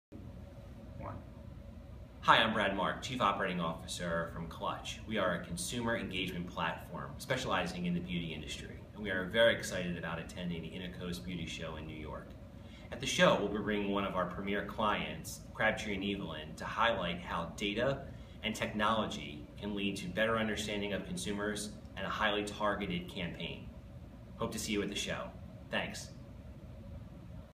How Consumer Management Technology Nurtures Crabtree & Evelyn's Customer Relationships
Join innoCos Digital conference and beauty20 Awards in New York on November 5

This session by Brad Marg, COO, Clutch.com and Russ Benblatt, Head of Marketing, Crabtree & Evelyn, will focus on a challenge faced by every brand: how to transform everyday interactions into enduring customer relationships.
Specifically, we will highlight the big consumer data problem that premier brands face and how Crabtree & Evelyn uses a one-of-a-kind customer engagement platform to communicate with customers on a personal level.
• Data Centralization
• Customer Intelligence
• Personalized Engagement
• Optimization